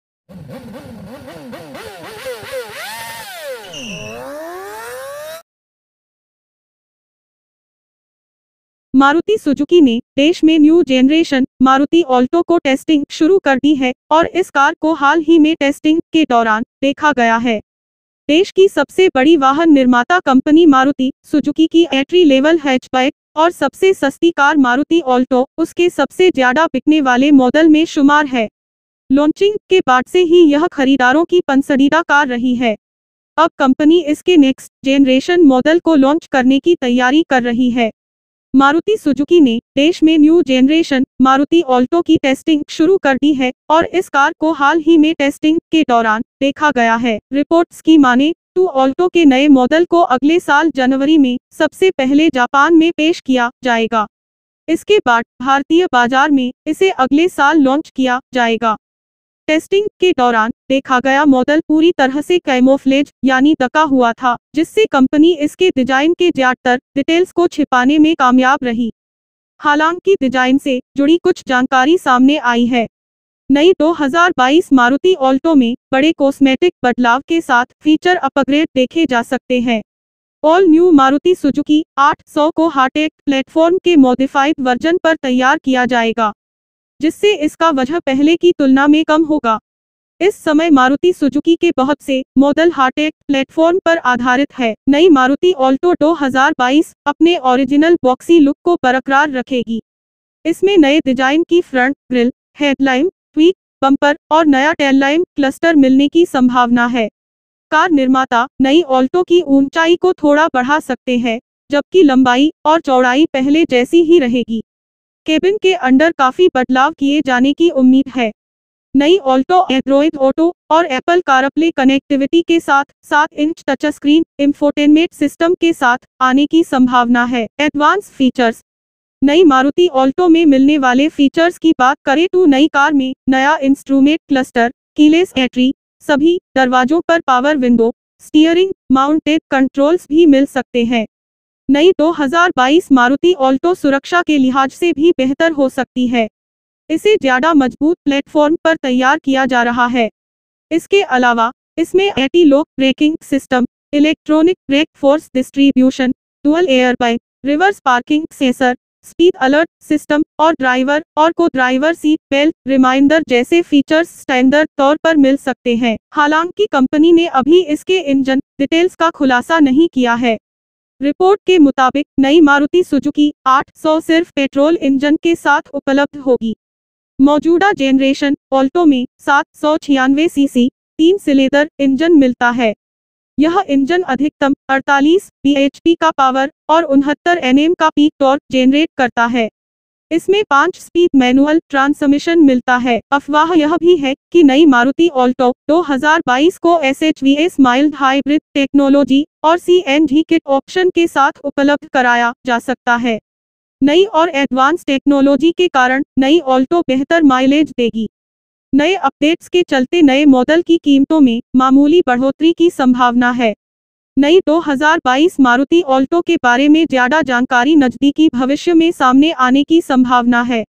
2022 Maruti Alto: आ रही है न्यू जेनरेशन मारुति ऑल्टो, मिलेगी एडवांस टेक्नोलॉजी और फीचर्स
Video Details :-